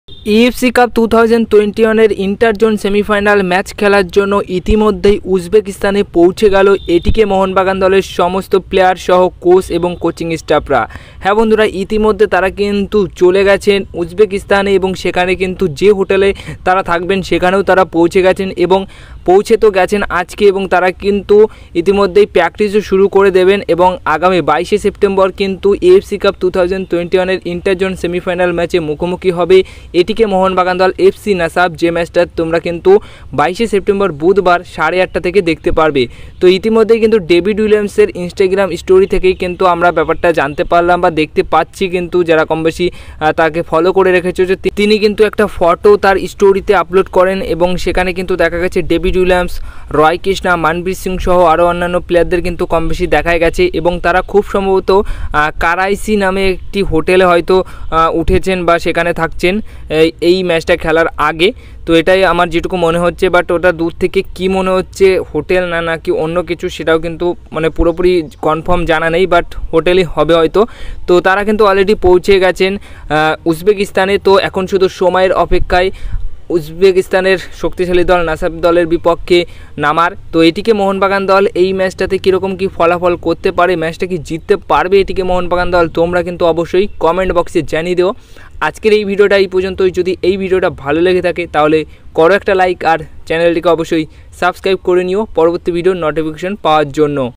উজবেকিস্তানে পৌঁছে গেল ATK MOHUN BAGAN দল | কঠিন ম্যাচের জন্য সবাই পৌঁছে গেল উজবেকিস্তানে🔥
🔵🔽photo collect(Image) :- Google Chrome